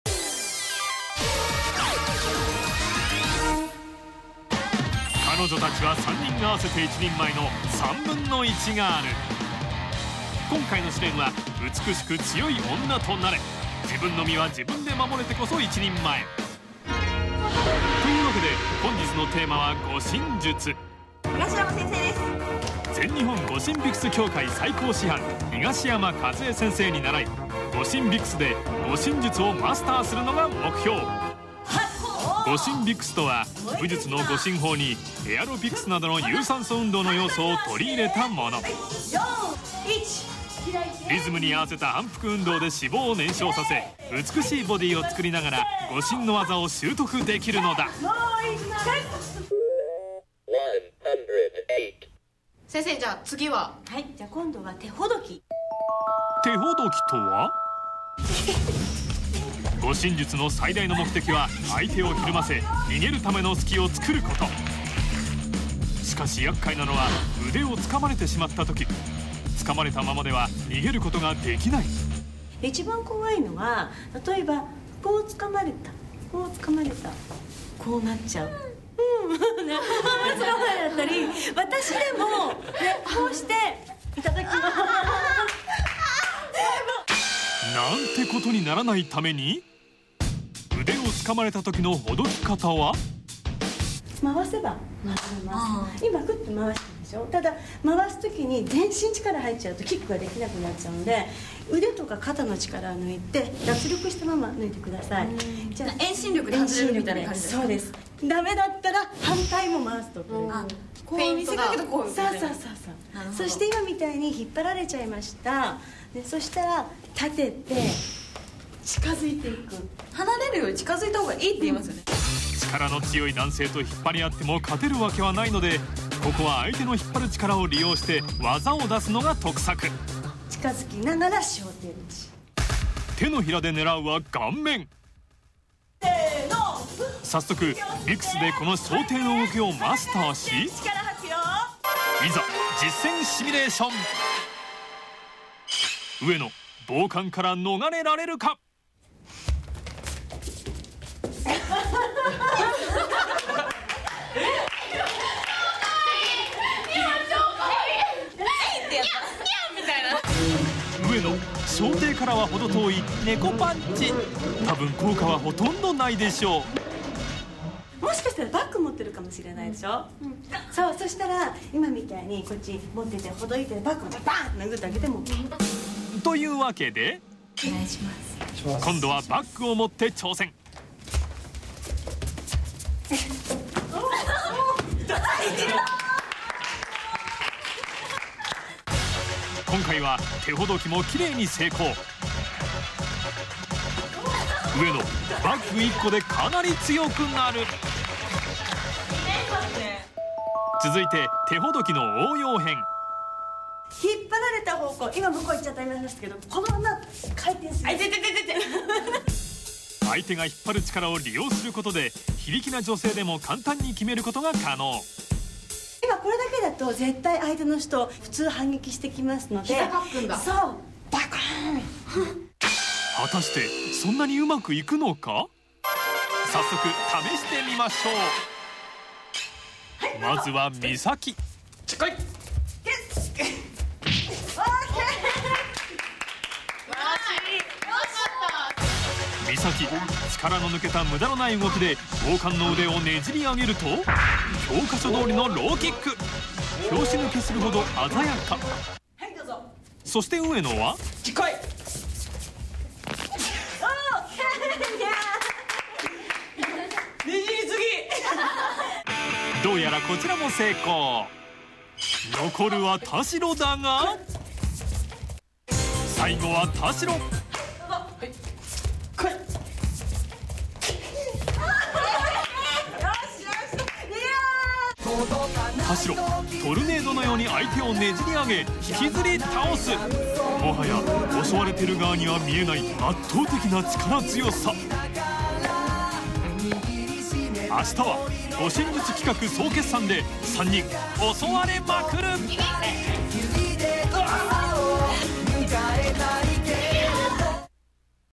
[1/3GIRL] Protect yourself Part.3
Three young girls Ayame, Sayaka and Mami, were ordered to learn how to protect themselves.